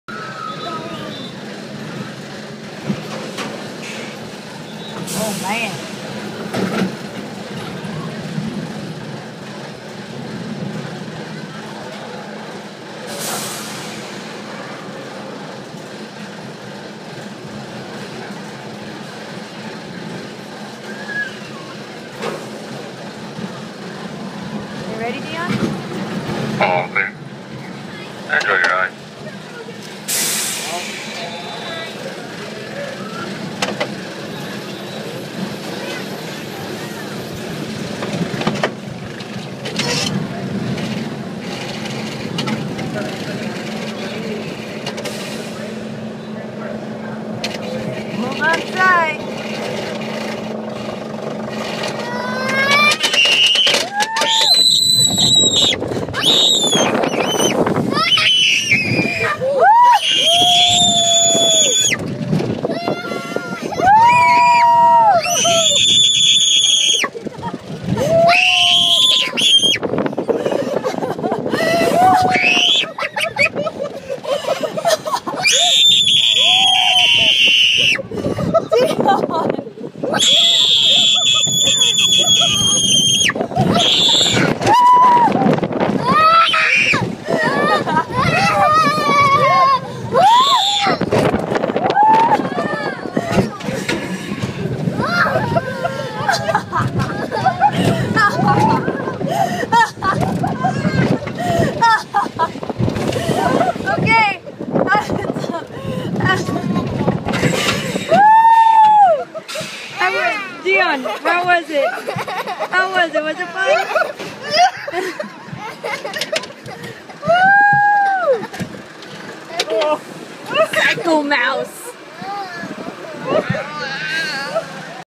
Psycho Mouse Ride (Great America)
Dionne screaming on Psycho Mouse ride at Great America in Santa Clara. Not her first time and still scared/excited to ride! With Jordan, Dante, and Mom.